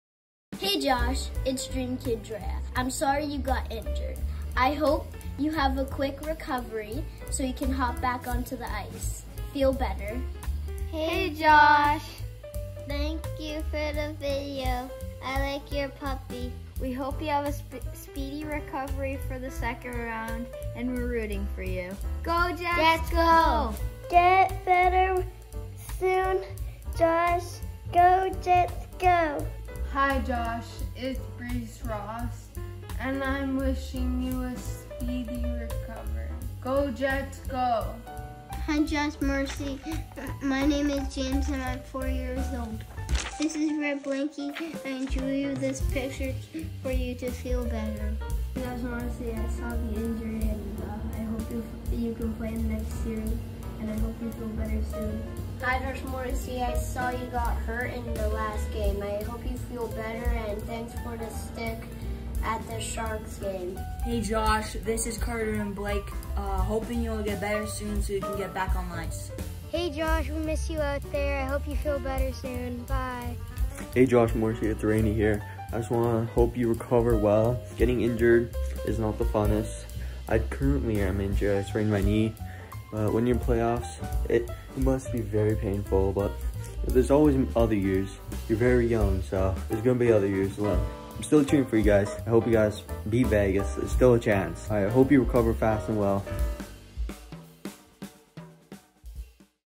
Dream Kids Say "Get Well Soon" to Josh Morrissey
Dream Kids Drea, Cody W., Maya, Breese, James, Cayden, Carter, Cody D., and Rainey wish Winnipeg Jets Dream Ambassador Josh Morrissey a speedy recovery during the Jets' playoff run in April, 2023.